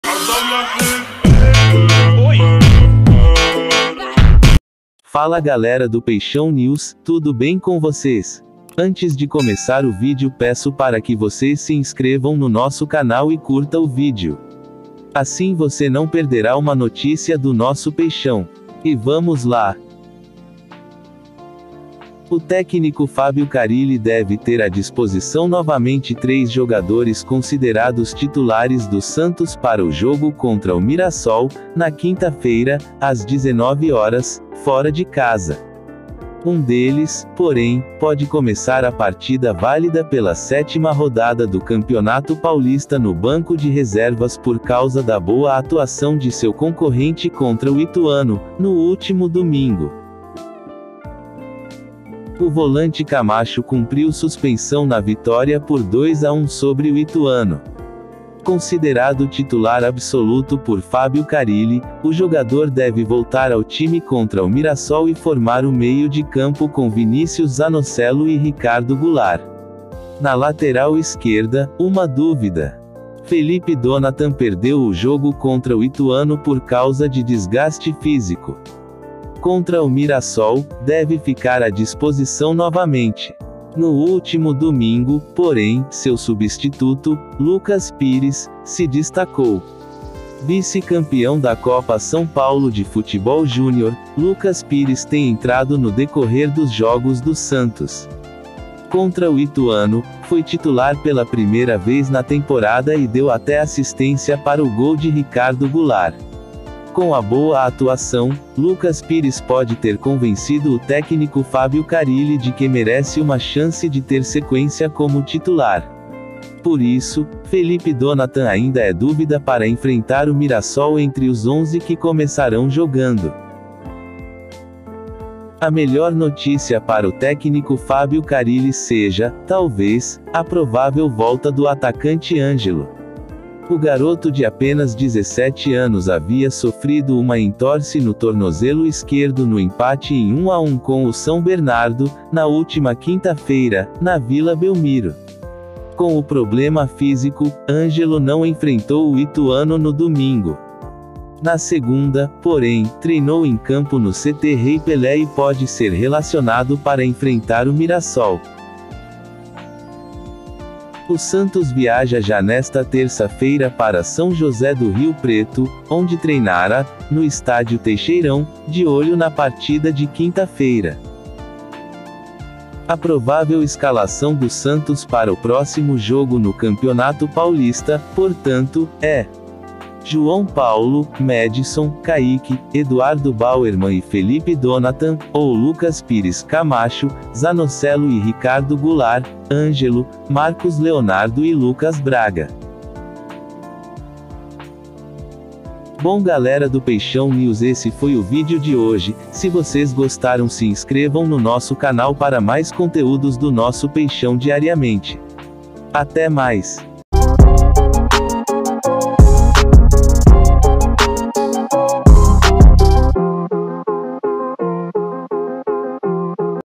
🔥🤍 TUDO O QUE VOCÊ PRECISA SABER PARA O JOGO ENTRE MIRASSOL X SANTOS
ESCALAÇÃO DEFINIDA, DESFALQUES E MAIS. TUDO O QUE VOCÊ PRECISA SABER PARA O JOGO ENTRE MIRASSOL X SANTOS